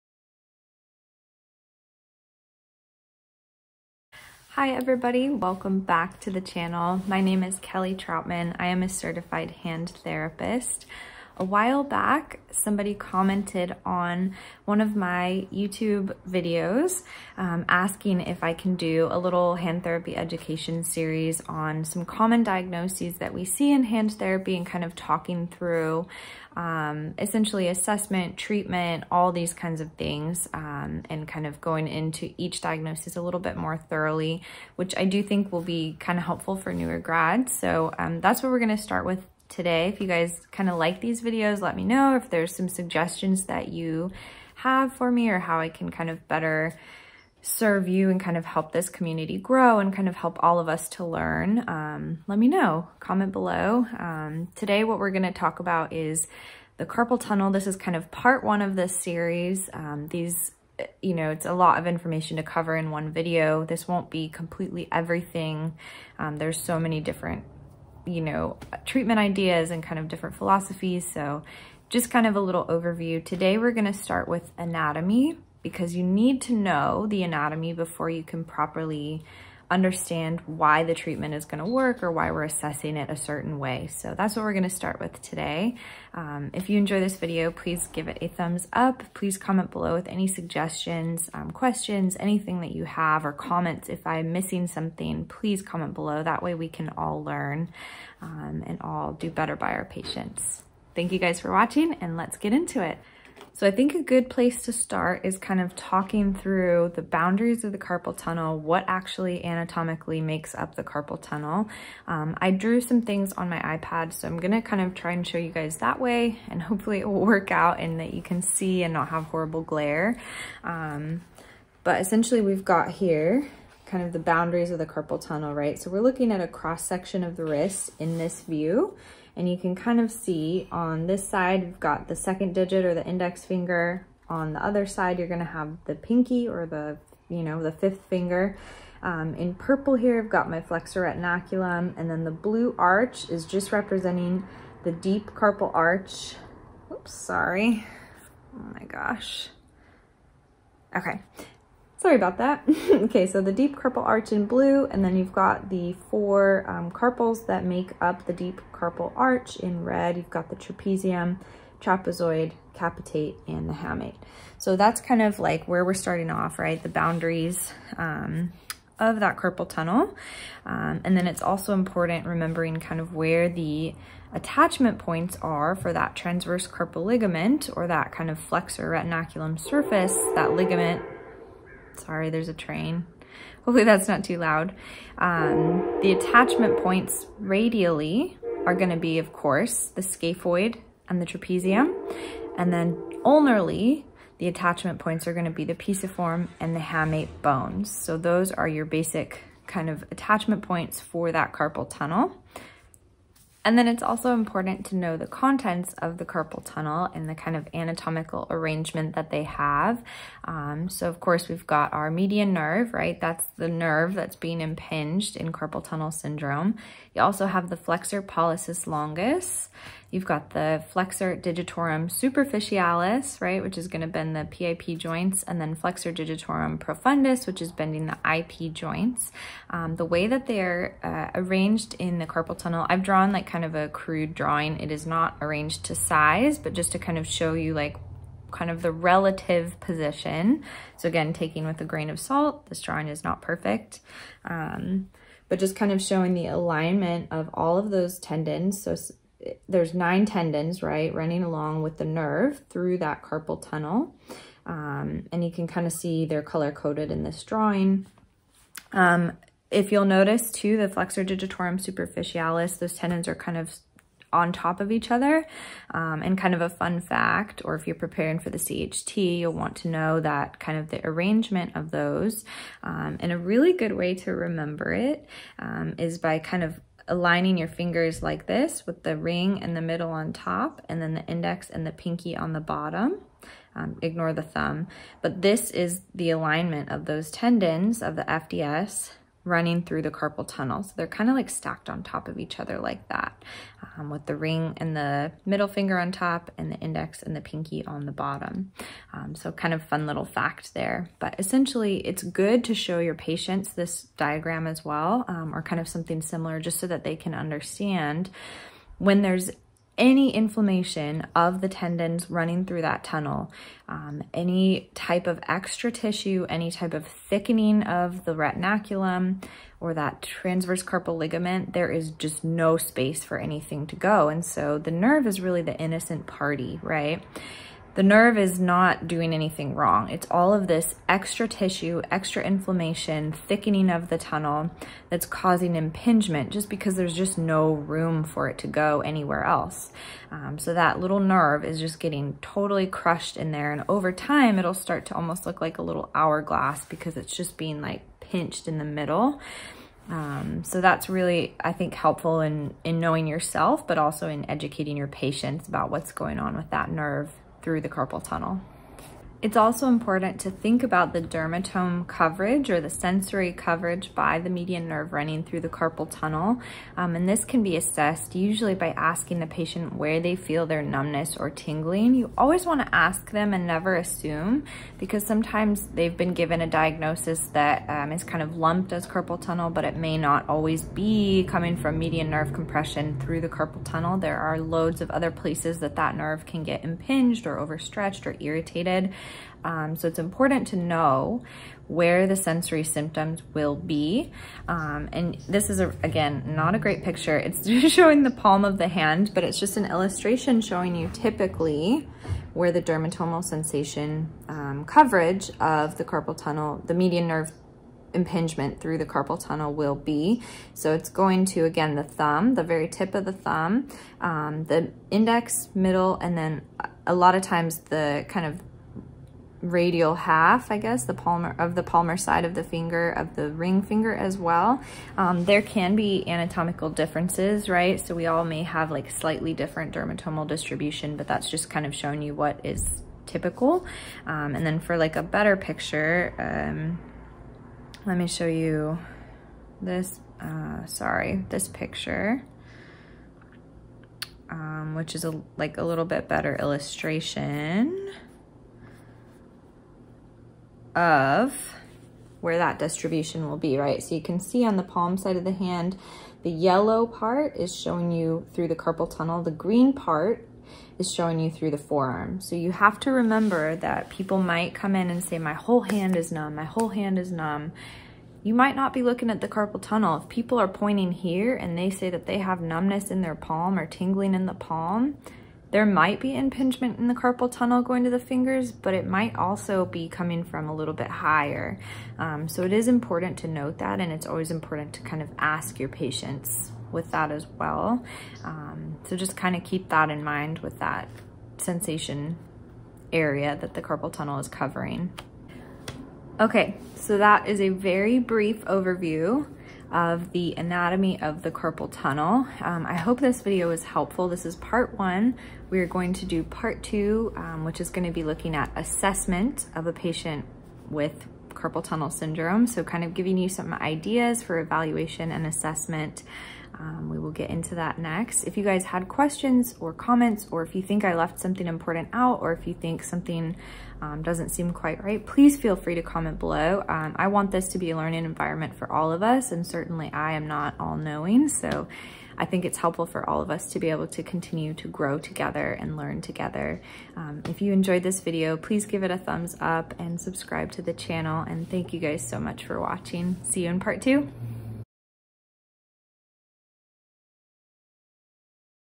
Learning Hand Therapy - Carpal Tunnel Syndrome Part I: Anatomy
This video is the first part in a hand therapy education series featuring common hand therapy diagnoses and how to assess / treat them (an open discussion, DEFINITELY not claiming I am all-knowing)! I remember being a fieldwork student and a new grad with VERY few resources and having NO idea how to assess or treat hand therapy diagnoses, so that's what I'm trying to do with this series!
This first video is the basic anatomy of carpal tunnel syndrome. This is meant to be a discussion, not a lecture (: